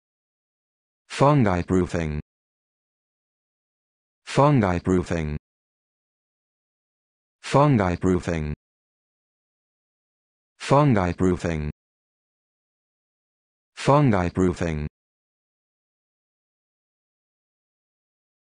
How to Pronounce Fungiproofing
This video shows you how to pronounce Fungiproofing